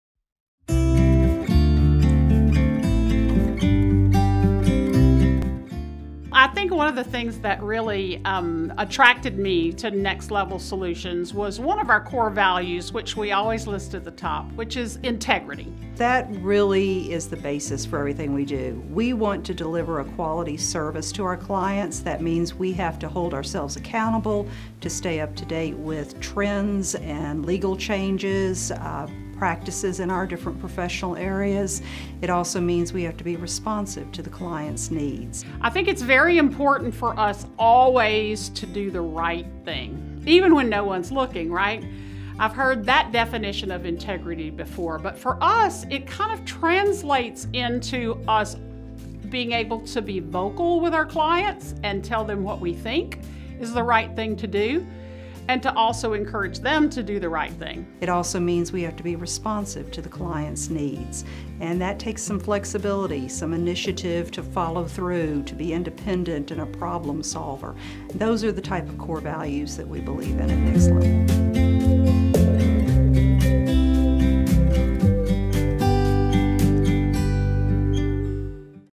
The Core Values Behind Next Level Solutions
At Next Level Solutions, our core values are evident in everything we do. We work to deliver quality services to our clients, and our beliefs are present across every step.